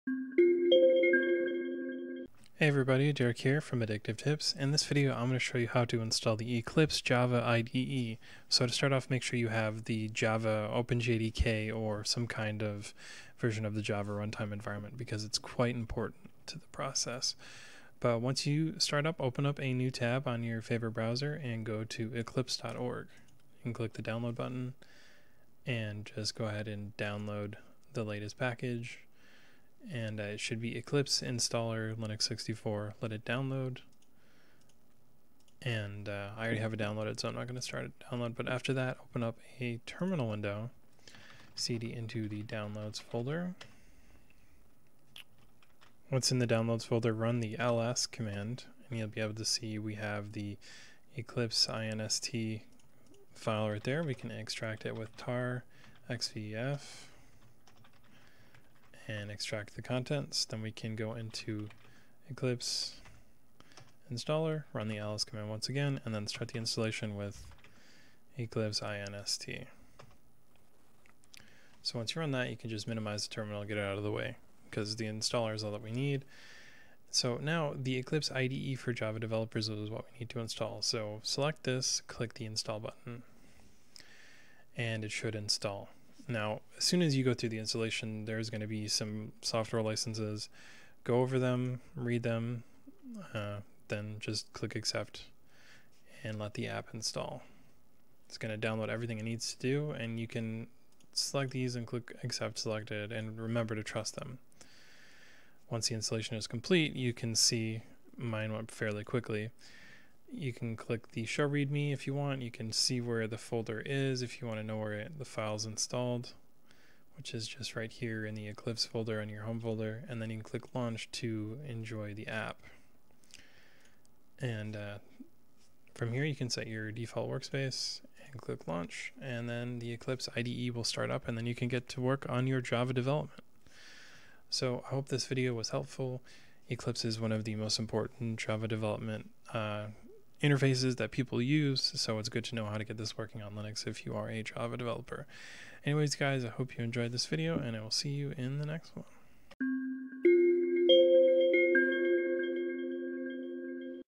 How to install the Eclipse Java IDE on Linux
Eclipse is the leading Java IDE for developers on Linux. It’s not hard to see why, as the app is filled with dozens of features, including support for Java, JSF, various XML editing tools, and even Git source control integrations.